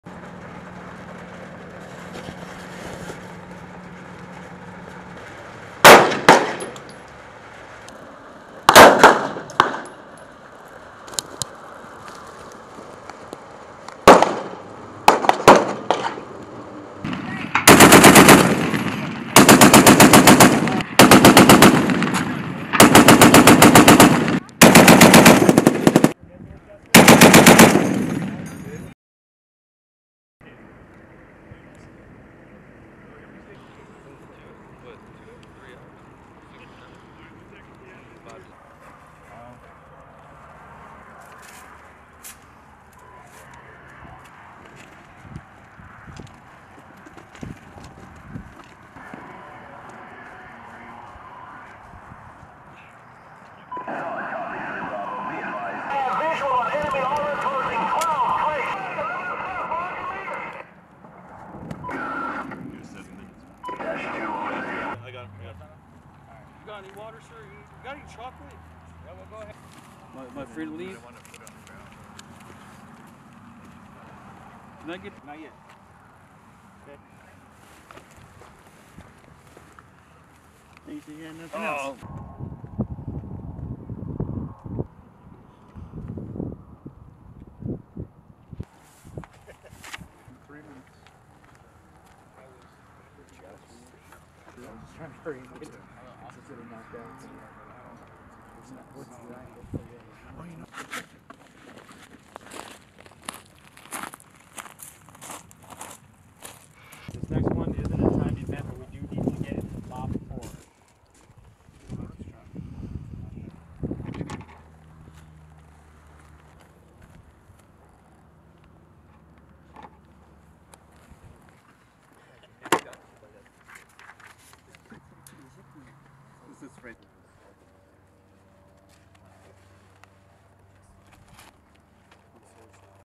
Arizona National Guard Best Warrior Competition 2024: Soldier’s Skill and Readiness
March 7, 2024 - Florence Military Reservation, Arizona: Soldiers from various units within the Arizona National Guard participate in the Best Warrior Competition 2024.

Soldier’s Skill and Readiness Shine at Arizona National Guard Best Warrior Competition 2024 B-Roll
FLORENCE, AZ, UNITED STATES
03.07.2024

The competition features rigorous training exercises, including weapons qualification, medical training, and land navigation. Participants demonstrate their dedication to readiness and excellence as they strive to showcase their skills and readiness. Additionally, the Arizona National Guard reaffirms its commitment to the State partnership Program with Kazakhstan, fostering enduring alliances and strengthening regional security since 1993.

Film Credits: Video by Spc. Justin Fallon
123rd Mobile Public Affairs Detachment